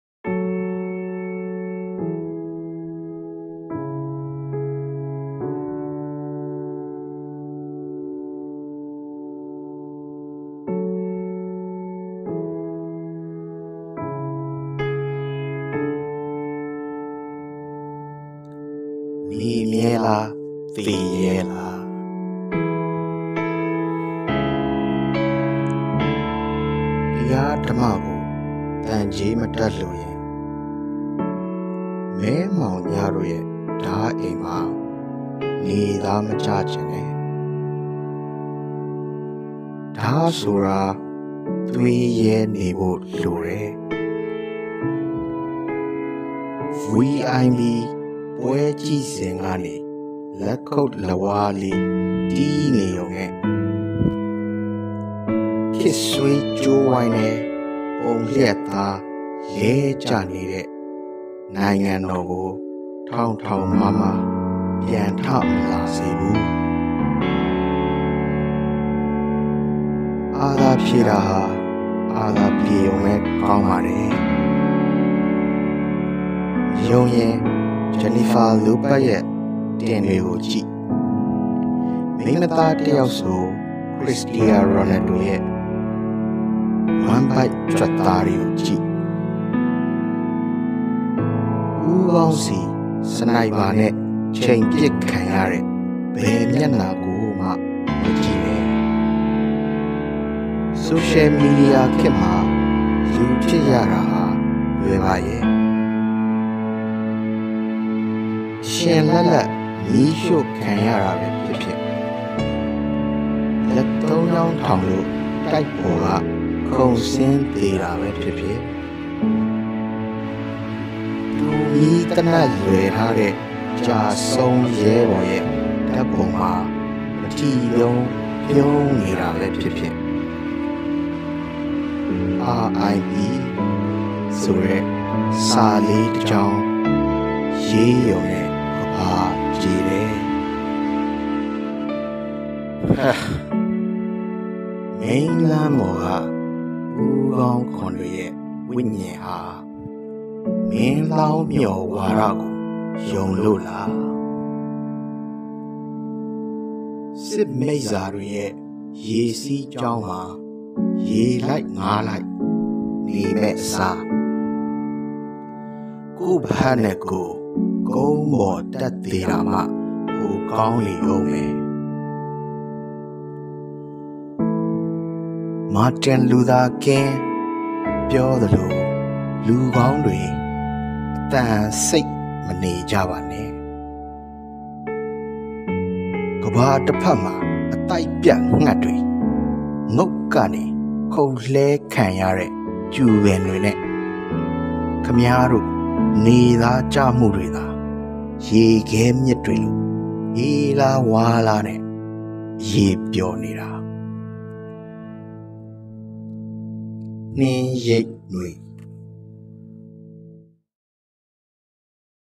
Nay Myal Lar Thay Yae Lar - နေမြဲလား သေရဲလား (Luu Ngal Entertainment)
" နေမြဲလား သေရဲလား "
ကဗျာဆရာ - နှင်းရိပ်နွေ
တူမီးတော်လှန်ရေးစာစဉ် ကဗျာကဏ္ဌမှထုတ်နုတ်၍လူငယ်အင်တာတိန်းမန့်ယူကျူချန်နယ်မှတင်ဆက်သည်။